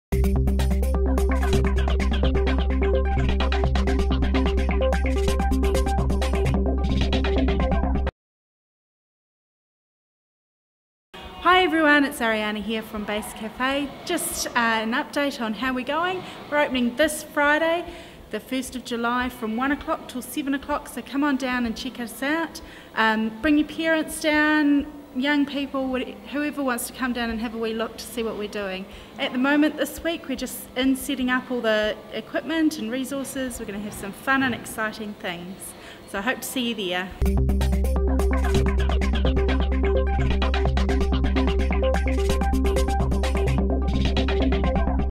BASE Youth Centre Update
We talk to Ariana Batstone about the reopening of Base Youth Centre this Friday on Tancred St.